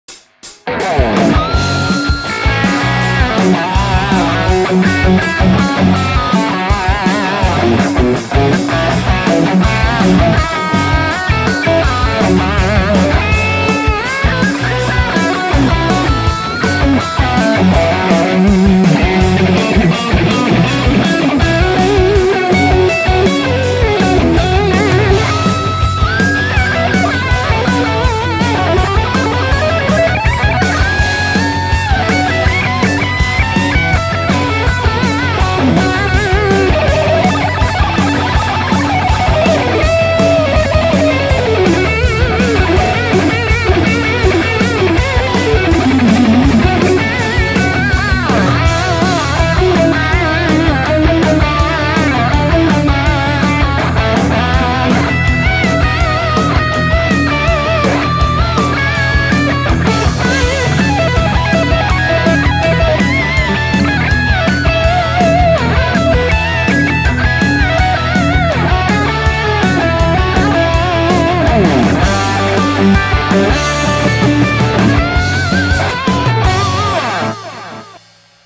Kemper profiling amplifier, Top Jimi Plexi 68 jam, JamTrackCentral, jam of the month
JTC august jam of the month, couple of mistakes, but hope you`ll like it! :)